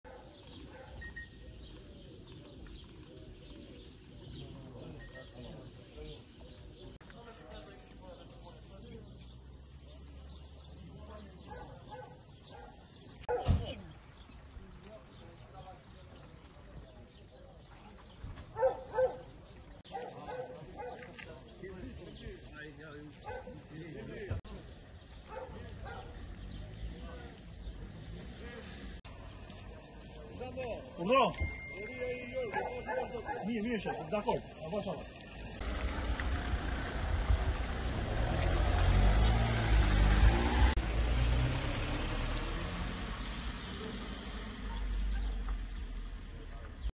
Top News/ Babai plagos me çifte djalin në Pogradec, e qëlloi në banesë pasi i biri e goditi
Rreth orës 18:40, në fshatin Stropckë, Pogradec, ka ndodhur  një konflikt familjar i  çastit  mes babait dhe djalit të  tij. Shtetasi P.Sh, 47 vjeç, në gjendje të dehur ka dhunuar babait e tij shtetasin A. Sh., 83 vjeç. Më pas 83 vjeçari ka marrë armën e gjahut dhe  ka plagosu  djalin e tij shtetasin P. Sh, i cili është transportuar për në spital për ndihmë mjekësore. Shërbimet e Komisariatit të Policisë Pogradec, kanë bërë  shoqërimin e 83 vjeçarit  në Komisariatin e Policisë për veprime të mëtejshme. Gjithashtu shërbimet e Policisë kanë bllokuar e sekuestruar në cilësinë e provës materiale armën e gjahut me të cilën dyshohet se 83 vjeçari ka plagosur djalin e tij. Grupi hetimor  në drejtimin e Prokurorisë po punon për zbardhjen e plotë të rrethanave dhe dokumentimin ligjor të kësaj ngjarjeje.